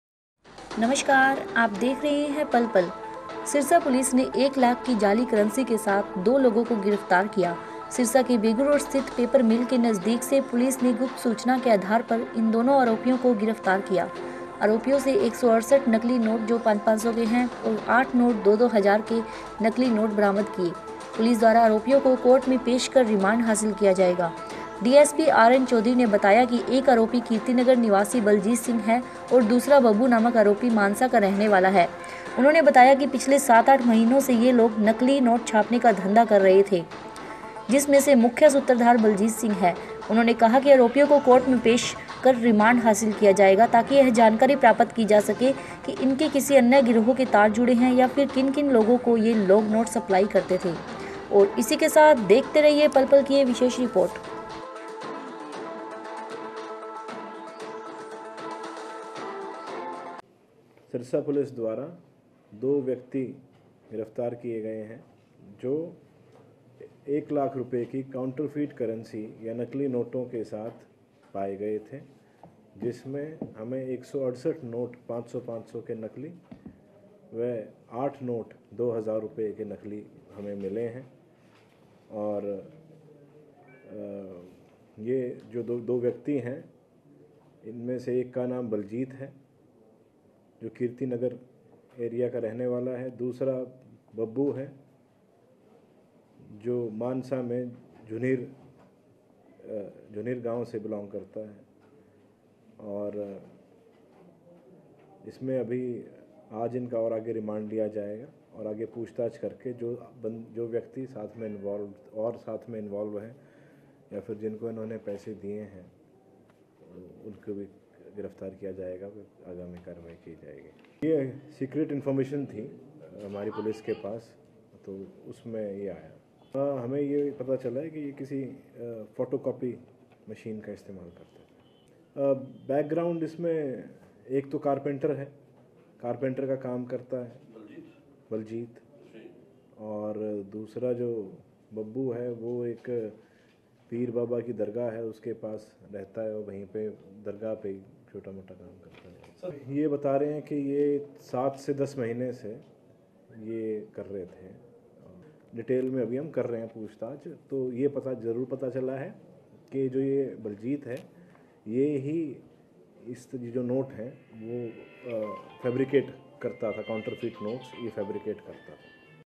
एक लाख की जाली करेंसी सहित दो लोग गिरफ्तार।
सिरसा पुलिस ने एक लाख की जाली करंसी के साथ दो लोगों को गिरफ्तार किया है . सिरसा के बेगू रोड स्थित पेपर मिल के नजदीक से पुलिस ने गुप्त सूचना के आधार पर इन दोनों आरोपियों को गिरफ्तार किया है .आरोपियों से 168 नकली नोट जो 500 500 के है व 8 नोट दो दो हजार के नकली नोट बरामद किए गए हैं। पुलिस द्वारा आरोपियों को कोर्ट में पेश कर रिमांड हासिल किया जायेगा।